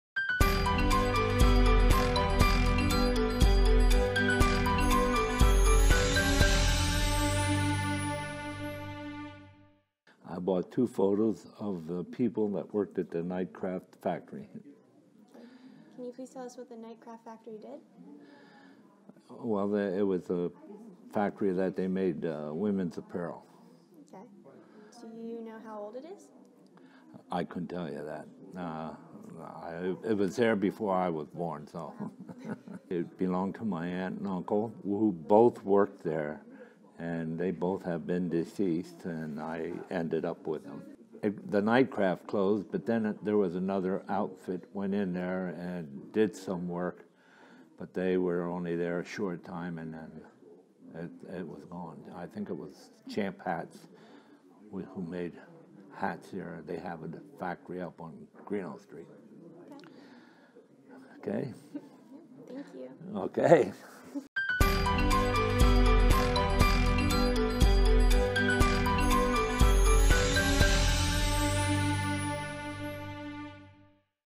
Edward Scholl History Harvest Interview
Interview with Edward Scholl conducted during the Sunbury History Harvest, April 8, 2017